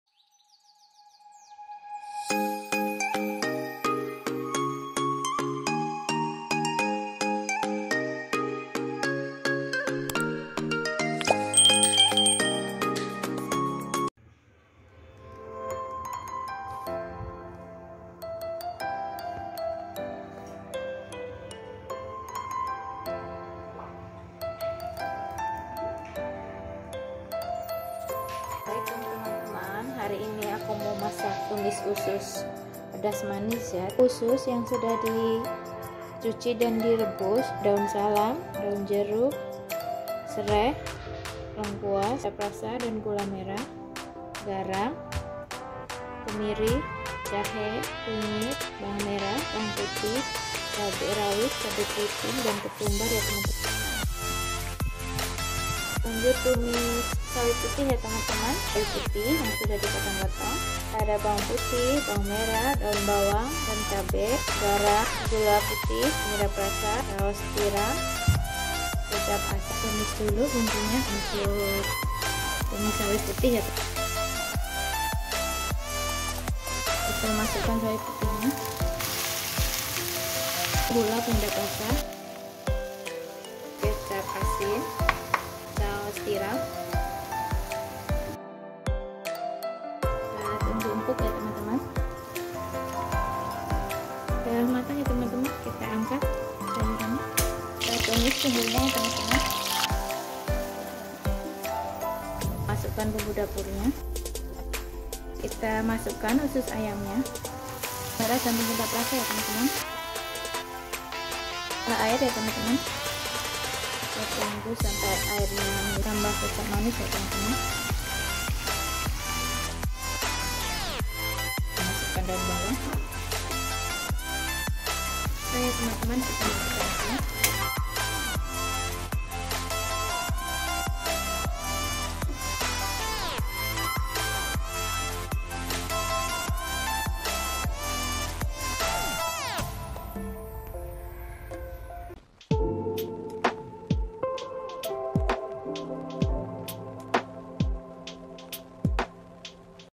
Menu Harian (Part 10) TUMIS USUS AYAM PEDAS MANIS DAN TUMIS SAWI PUTIH
📌Bahan dan bumbu tumis usus :
     *Usus
     *Daun bawang
     *Cabe rawit,cabe keriting,bawang merah
       bawang putih,kemiri,ketumbar,jahe,kunyit
     *Daun salam,daun jeruk,sereh dan lengkuas
     *Kecap manis,air dan minyak goreng

📌Bahan dan bumbu tumis sawi:
     *Sawi putih
     *Daun bawang
     *Cabe,bawang merah dan bawang putih
     *Garam,gula,penyedap rasa,kecap asin dan
       saos tiram.
     *Air dan minyak goreng.

Pemusik: @iksonmusic

Pemusik: @iksonmusic